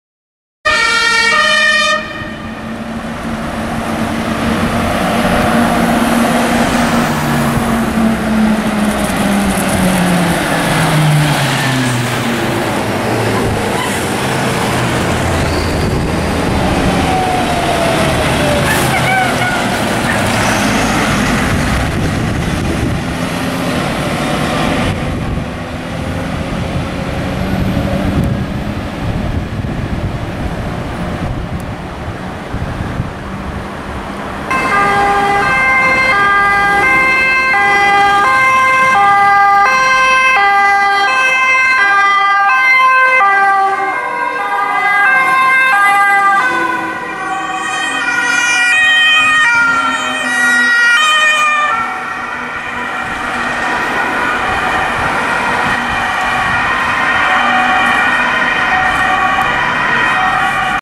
Löschzug FW2 & Löschzug mit Doppel RTW FW1 BF + FuStKW Polizei Hagen
Zu zwei unterschiedlichen Brandmeldealarmen rücken von der Feuer- und Rettungswache 2 der Löschzug sowie von der Feuer- und Rettungswache 1 der Löschzug mit zwei Rettungswagen aus. Zusätzlich folgt ein FuStKW der Polizei Hagen den Fahrzeugen der Feuer- und Rettungswache 1.